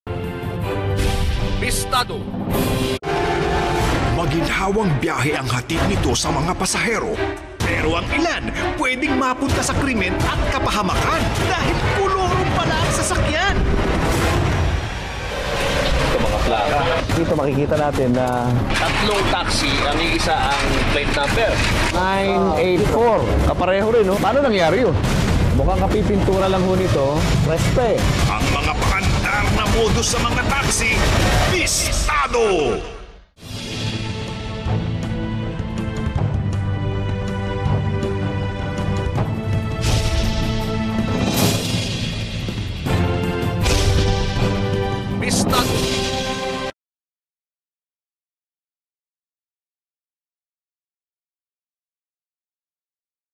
Double plate number, busted!
Beware of modus operandi on taxi. Watch the video for more information!